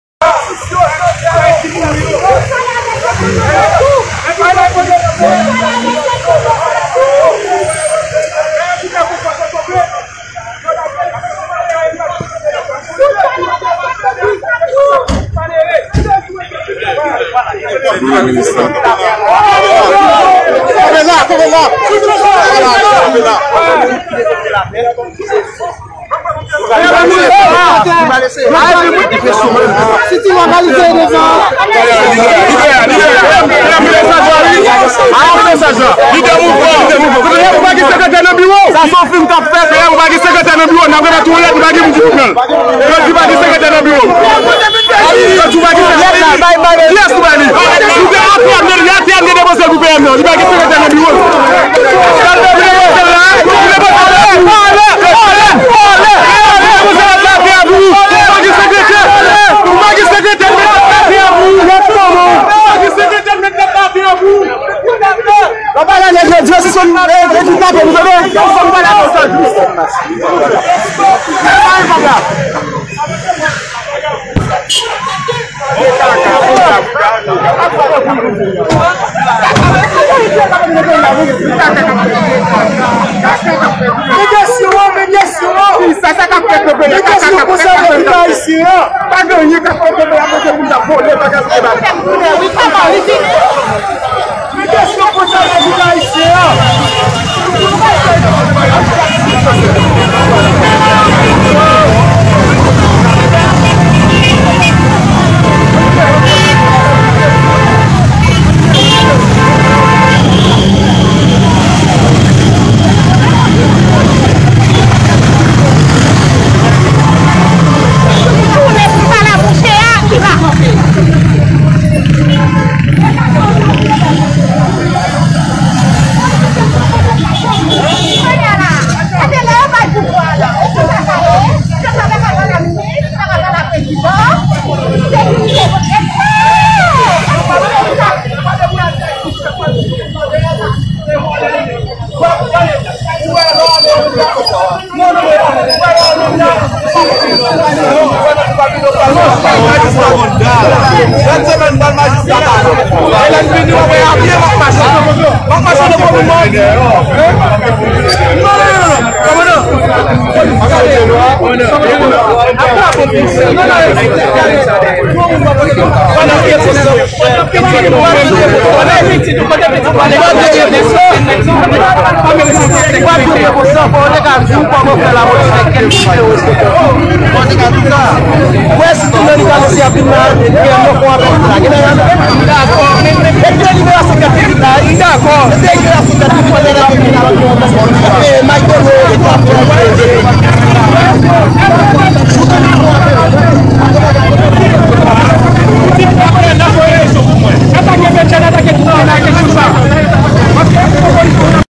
Sit-In Kont Kindnaping: PM Joseph Jouthe Desann Machinn li pou Pale ak Manifestan yo devan lakay li
PM Joseph Jouthe Desann Machinn li pou Pale ak Manifestan yo devan lakay li.